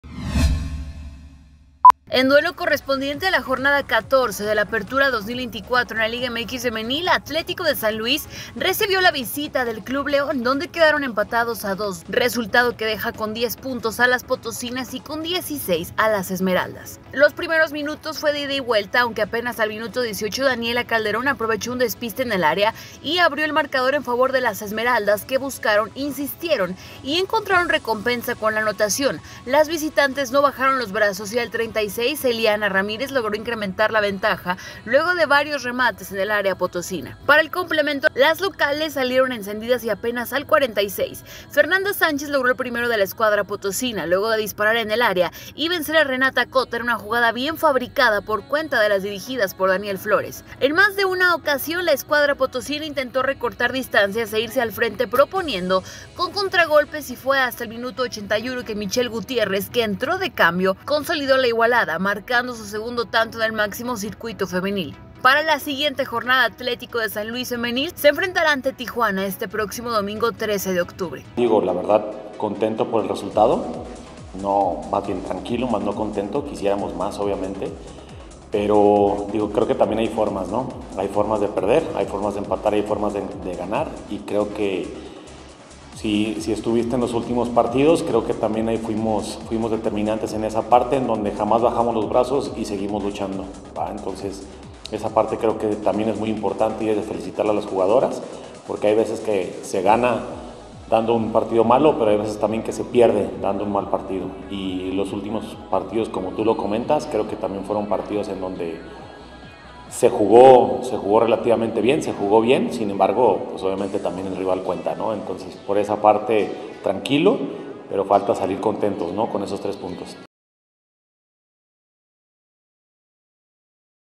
Atlético de San Luis Femenil rescata empate ante León
SLP.- En duelo correspondiente a la Jornada 14 del Apertura 2024 en la Liga MX Femenil, Atlético de San Luis recibió la visita del Club León, donde quedaron empatados a 2 resultado que deja con diez puntos a las potosinas y con 16 a las Esmeraldas.

Los primeros minutos fue de ida y vuelta, aunque apenas al minuto 18, Daniela Calderón aprovechó un despiste en el área y abrió el marcador en favor de las esmeraldas, que buscaron, insistieron y encontraron recompensa con la anotación. Las visitantes no bajaron los brazos y al 36’, Elliana Ramírez logró incrementar la ventaja, luego de varios remates en el área potosina.

Para el complemento, las locales salieron encendidas y apenas al 46’, Fernanda Sánchez logró el primero de la escuadra potosina, luego de disparar en el área y vencer a Renatta Cota, en una jugada bien fabricada por cuenta de las dirigidas por Daniel Flores.

En más de una ocasión, la escuadra potosina intentó recortar distancias e irse al frente, proponiendo con contragolpes y fue hasta el minuto 81, que Michelle Gutiérrez, que entró

de cambio, consolidó la igualada, marcando su segundo tanto en el máximo circuito femenil.

Para la siguiente jornada, Atlético de San Luis Femenil se enfrentará ante Tijuana, este próximo domingo 13 de octubre.